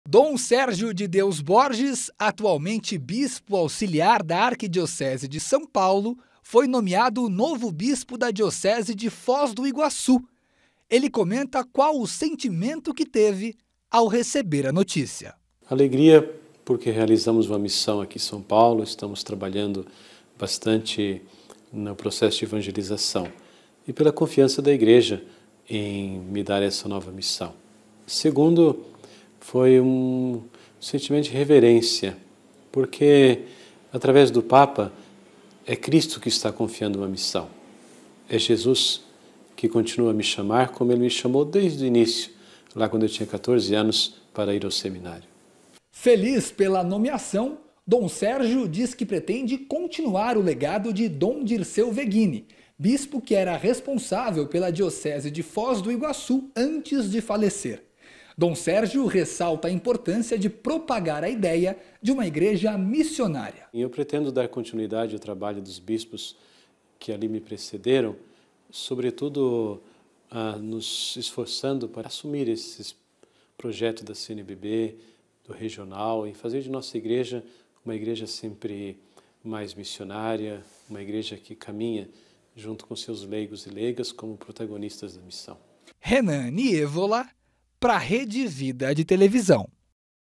Dom Sérgio Borges é nomeado bispo de Foz do Iguaçu - JCTV - 17/07/2019
O jornalista Renan Nievola conversou com o bispo nomeado de Foz do Iguaçu, dom Sérgio de Deus Borges.